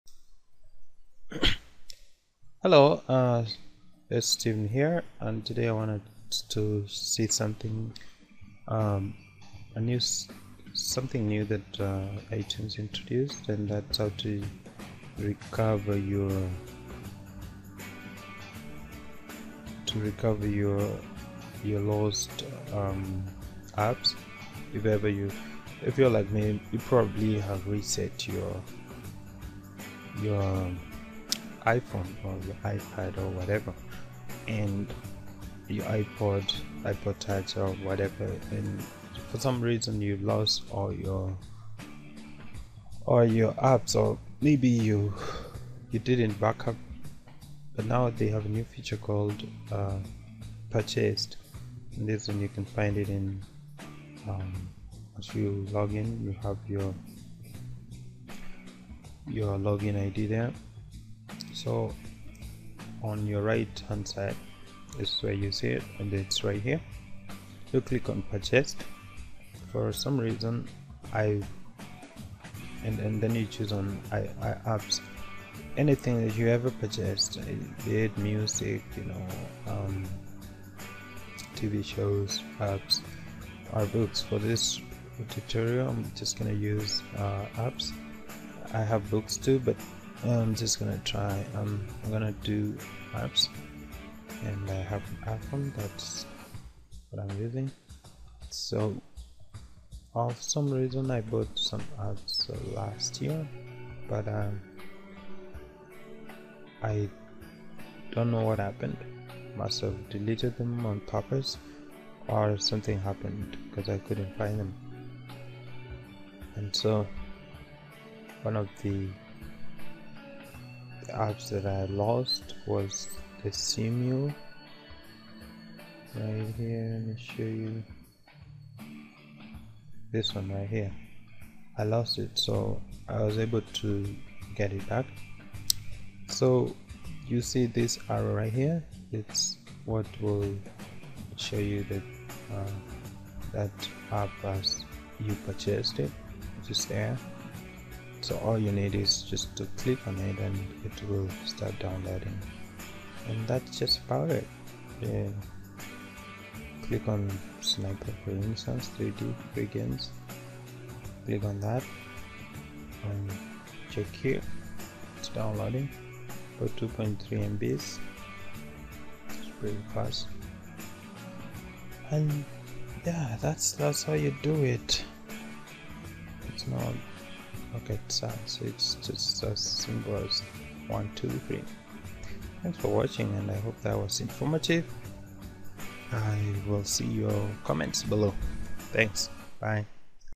HOW TO RECOVER LOST ITEMS IN iTunes
If you accidentally deleted an app, music, video or even books from iTunes, then this video should be an answer on how to recover that... enjoy/endure! :)